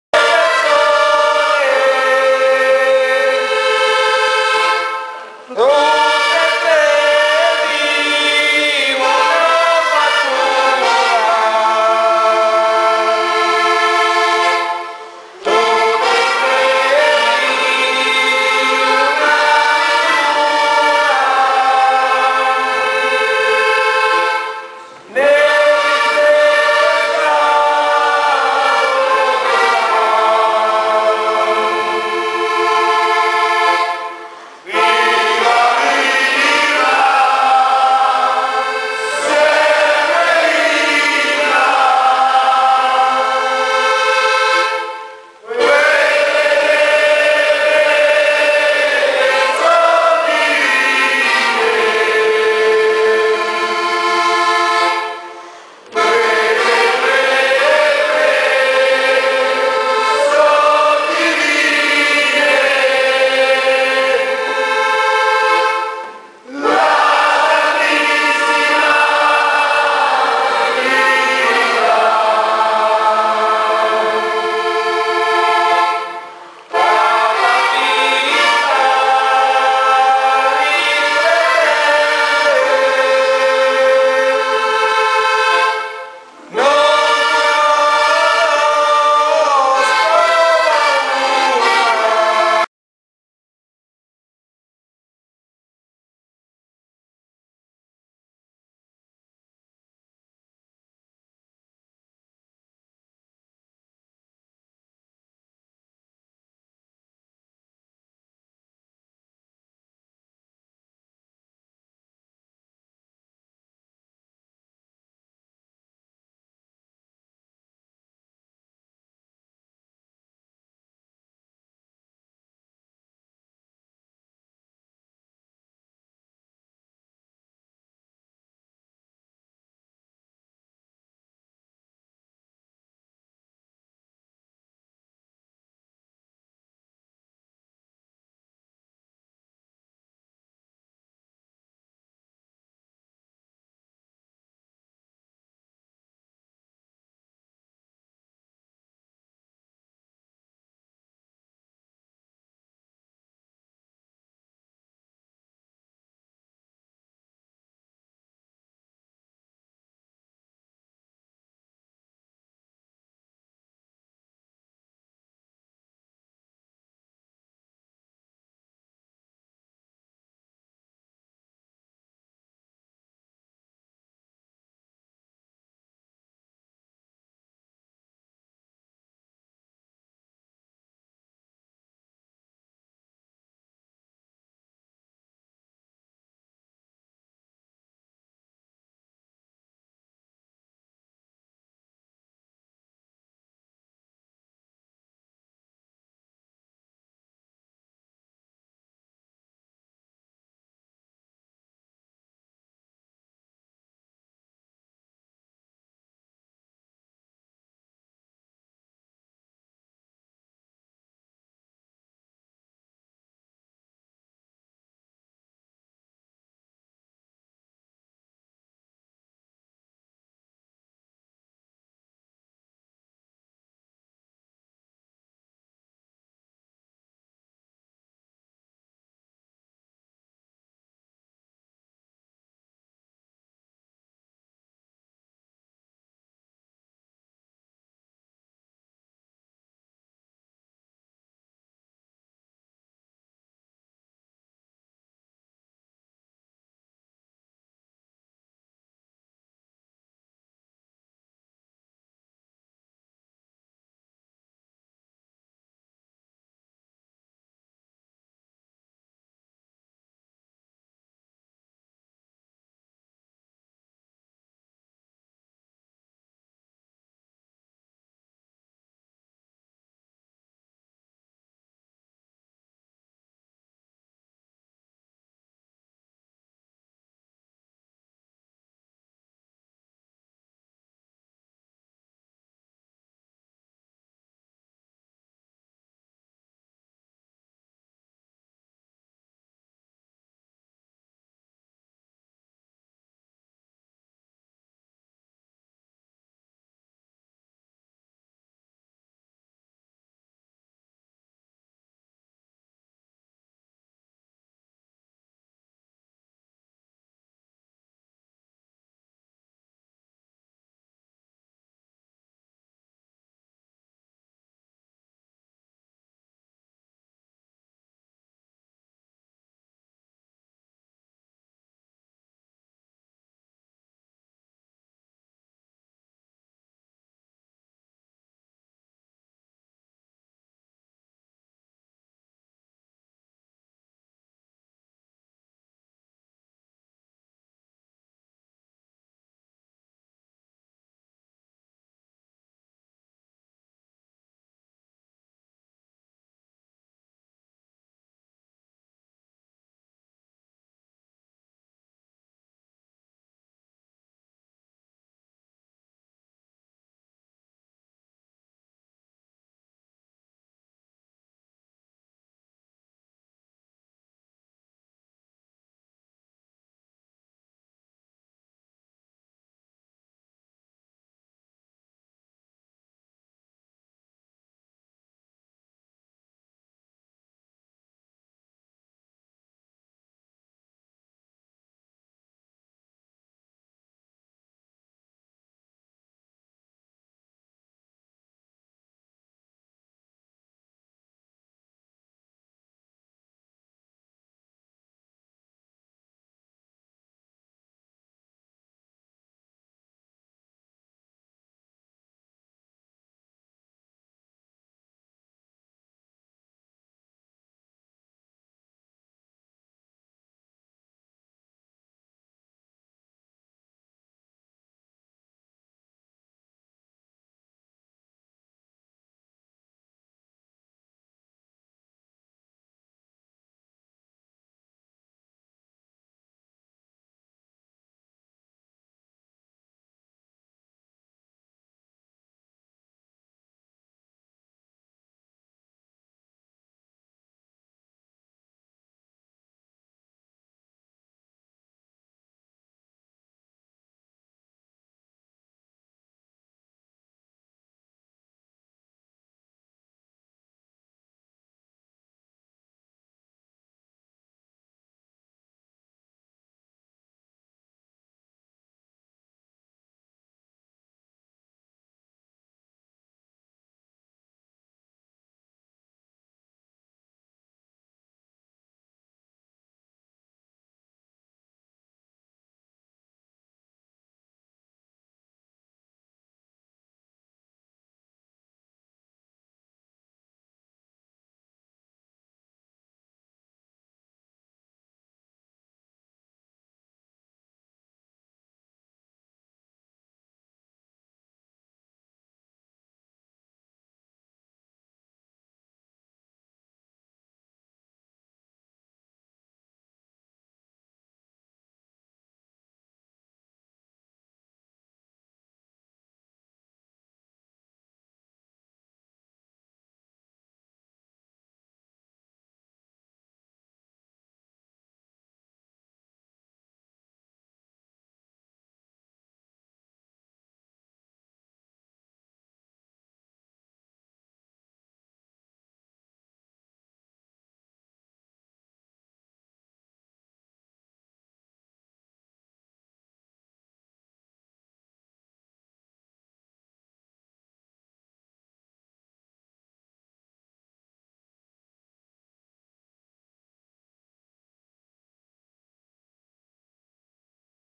vallepietra 2009
Un pellegrinaggio a piedi da La Lucca di Monte al Santuario SS.ma Trinità di Vallepietra, che con fede e devozione si rinnova dal 1996.